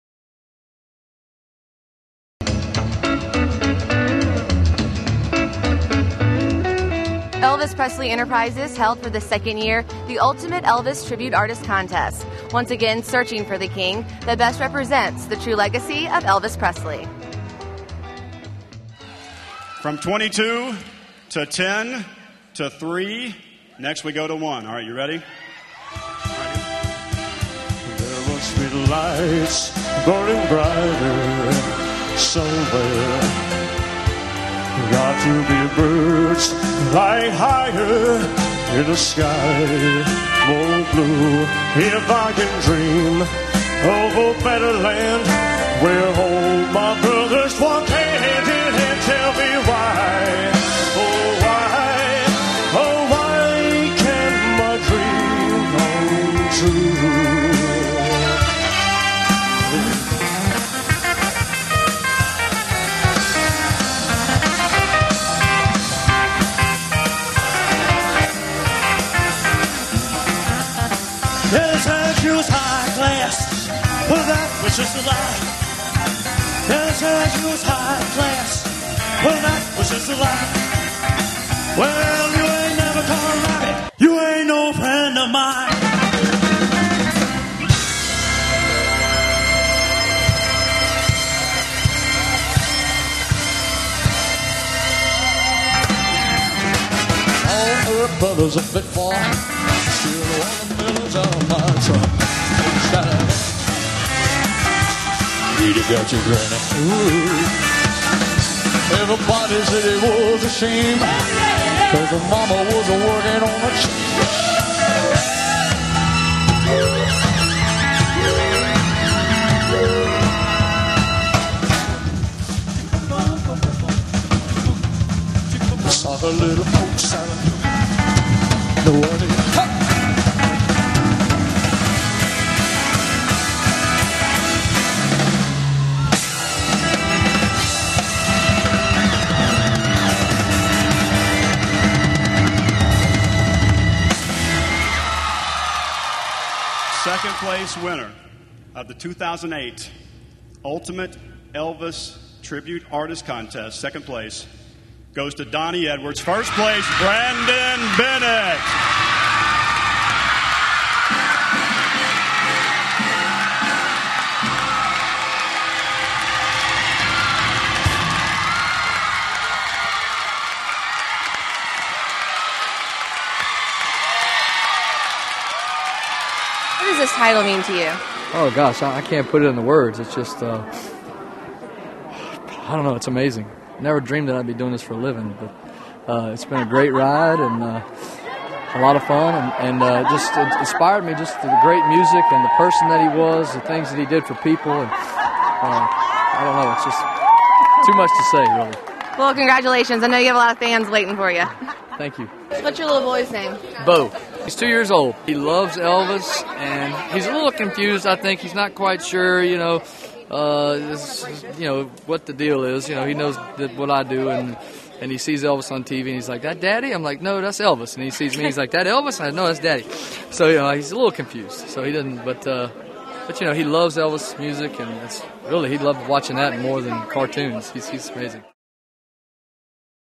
2008 Ultimate Elvis Tribute Contest
Brandon Bennett, from Ponchatoula, Louisiana was named the 2008 Ultimate Elvis Tribute Artist.